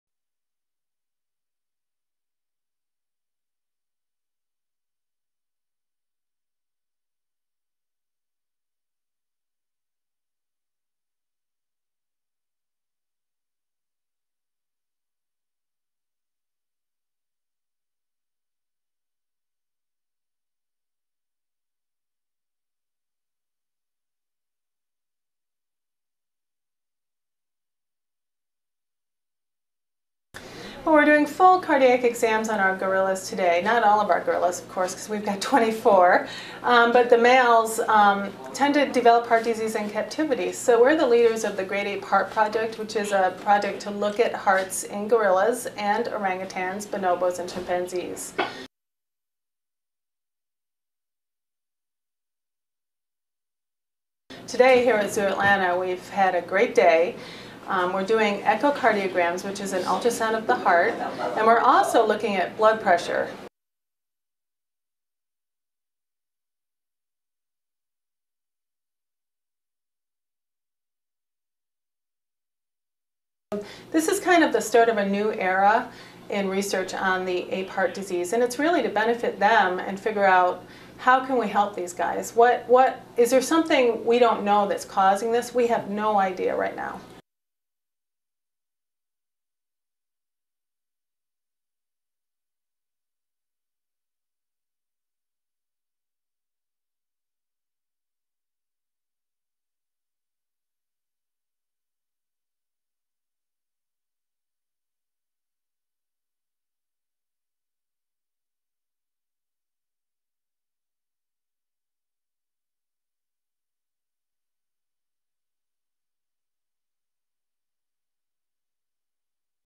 Heart examinations on male gorillas at zoo
++MUTE
1. Mid of 8-year-old male gorilla, Mbeli, lying down on examination table
2. Close-up of monitoring equipment in operating room
3. Mid of medics examining 22-year-old male gorilla Kekla
4. Wide of medics checking Kekla's finger mobility
5. Close-up of IV drip
6. SOUNDBITE (English) Dr. Hayley Murphy, Director of Veterinary Services, Zoo Atlanta:
"We're doing full cardiac exams on our gorillas today - not all of our gorillas of course, we have 24; but the males tend to develop heart disease in captivity. So, we're the leaders of the Great Ape Project, which is a project to look at hearts in gorillas and orangutans, bonobos and chimpanzees"
++MUTE
7. Close-up of syringes on tray
8. SOUNDBITE (English) Dr. Hayley Murphy, Director of Veterinary Services, Zoo Atlanta:
"Today, here at Zoo Atlanta, we've had a great day. We're doing echo cardiograms, which is an ultrasound of the heart, and we're also looking at blood pressure."
++MUTE
9. Pull out of Kekla's hand, with IV attached and monitors in the background, to wide of operating room and medics
10. SOUNDBITE (English) Dr. Hayley Murphy, Director of Veterinary Services, Zoo Atlanta:
"This is kind of the start of a new era in research on the ape heart disease and it's really to benefit them and figure out how can we help these guys, what, is there something we don't know that's causing this - we have no idea right now."
++MUTE
11. Close-up of Zoo Atlanta name tag on operating room technician
12. Wide of operating room staff preparing to remove Mbeli
13. Wide of operating room staff carrying Mbeli out after examination
14. Wide of Zoo Atlanta entrance sign
STORYLINE:
Medical experts at Zoo Atlanta in Georgia have been checking male great apes for heart problems, as part of a project to diagnose and treat heart disease.
Vets measured the animals' blood pressure and looked at the performance of their hearts using cardiac ultrasounds.
The examinations took place as part of the Great Ape Heart Project -  which is looking at the health of apes living in captivity.
According to experts, male gorillas looked after in zoos tend to develop cardiac problems.
"This is kind of the start of a new era in research on the ape heart disease and it's really to benefit them and figure out how can we help these guys, is there something we don't know that's causing this - we have no idea right now," Dr. Hayley Murphy from Zoo Atlanta said.
Zoo Atlanta is home to the United States' largest collection of western lowland gorillas.